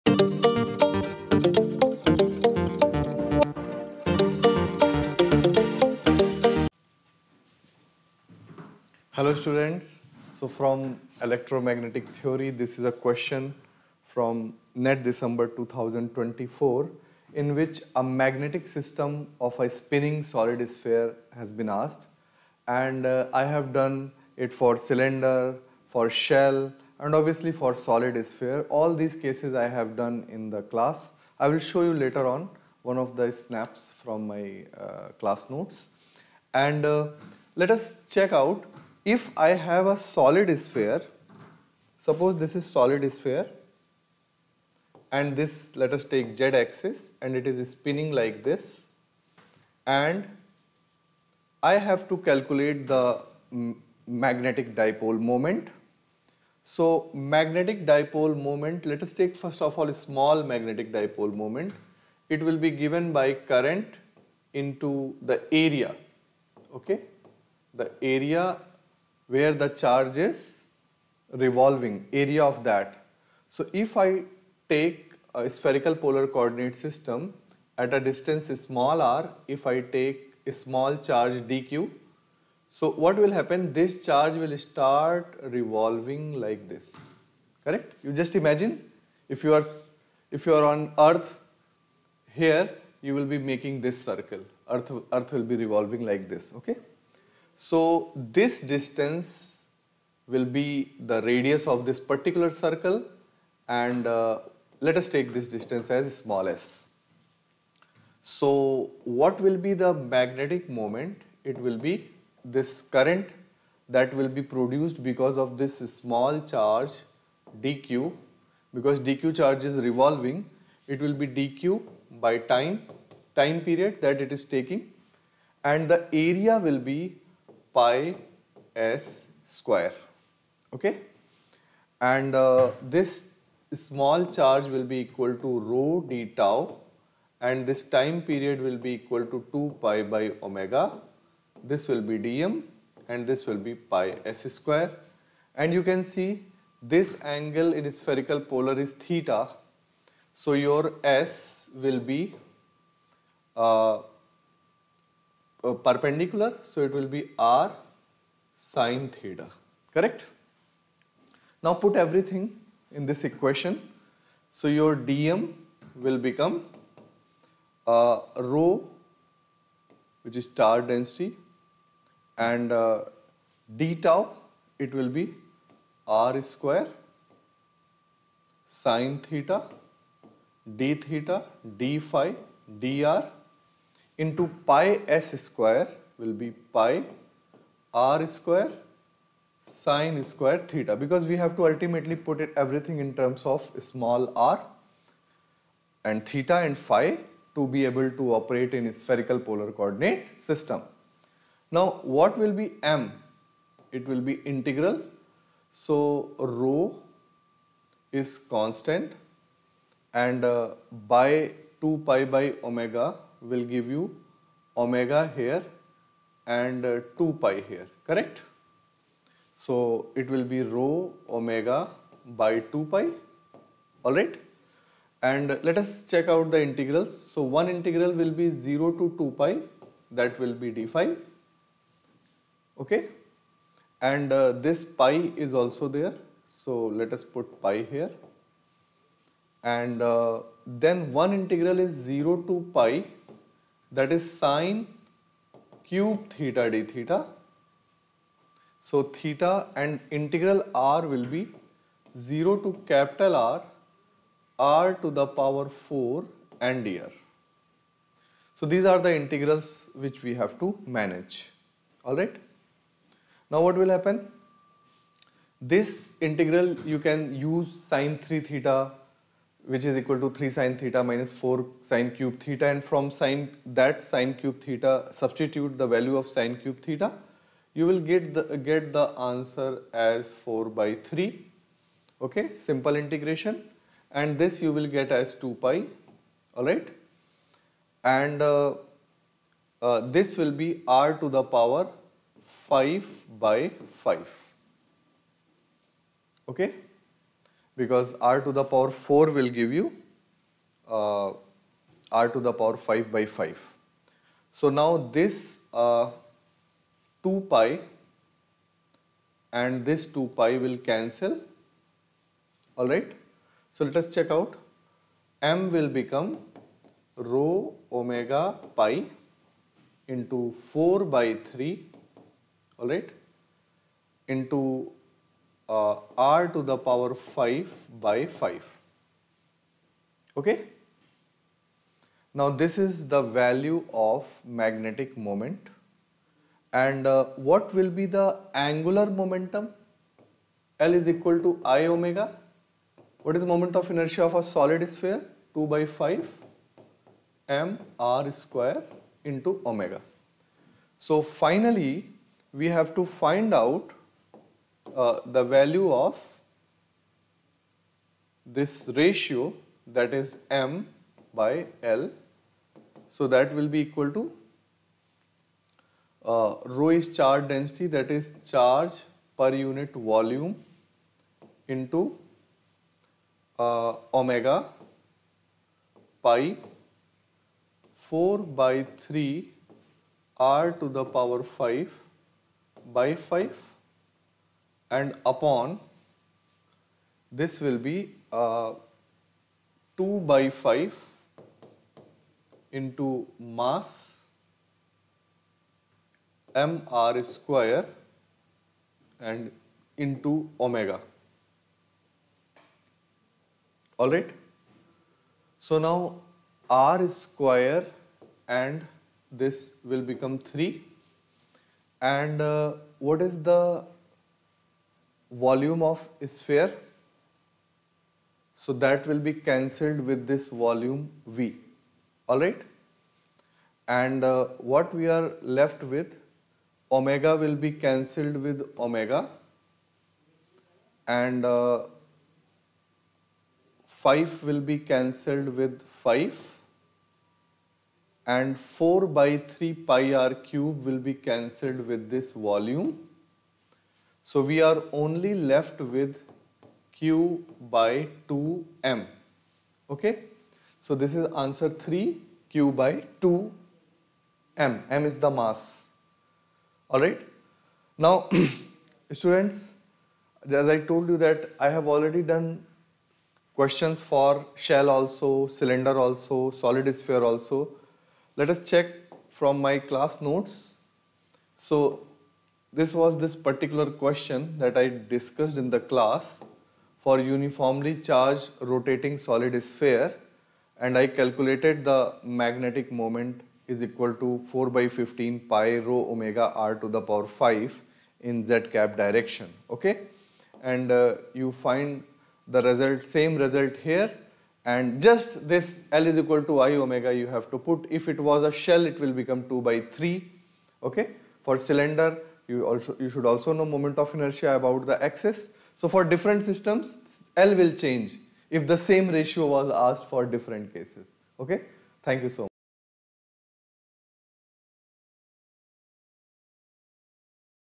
CSIR NET Dec 2024 | QID 705136 | EMT Magnetic Moment Solution by Alok Sir | Pravegaa Education
🔹 CSIR NET Dec 2024 | QID 705136 | Electromagnetic Theory (EMT) – Magnetic Moment Solution by Alok Sir
In this detailed solution video, Alok Sir from Pravegaa Education explains the concept of Magnetic Moment in Electromagnetic Theory (EMT), covering essential techniques and strategies to solve similar problems in CSIR NET Physics. This solution will enhance your problem-solving skills and strengthen your conceptual understanding for CSIR NET Dec 2024.

💡 Key Highlights:
✅ Step-by-step solution for QID 705136 from CSIR NET Dec 2024.
✅ Expert insights and tips by Alok Sir.
✅ Ideal for students preparing for CSIR NET, IIT JAM, and GATE Physics.
✅ Improve your accuracy and speed with detailed explanations.

Want to crack CSIR NET & IIT JAM Physics? Talk to Academic Expert